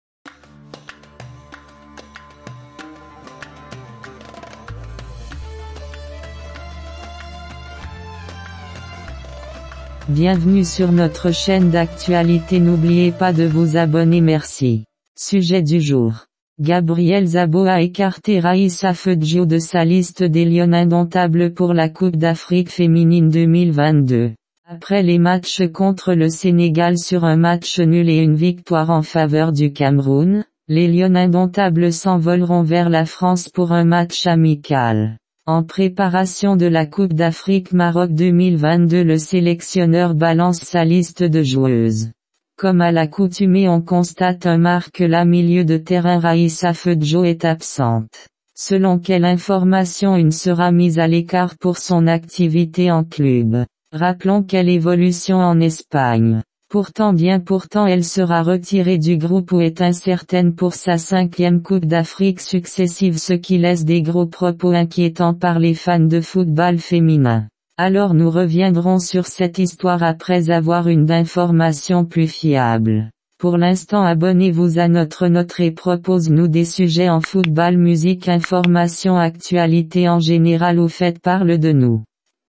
Can Féminine : Raïssa Feudjo mise à l’écart du groupe des lionnes indomptables pour la can 2022
Après deux matchs amicaux sur un nul et une victoire
Les lionnes s’envolent le 28 pour un match contre les bleues
Au compte d’un amicale de en préparation pour le Maroc

Le sélectionneur Zabo Gabriel écartela milieu Raïssa Feudjo
Qui ne jouera peut-être pas sa 5e coupe d’Afrique consécutive

Le se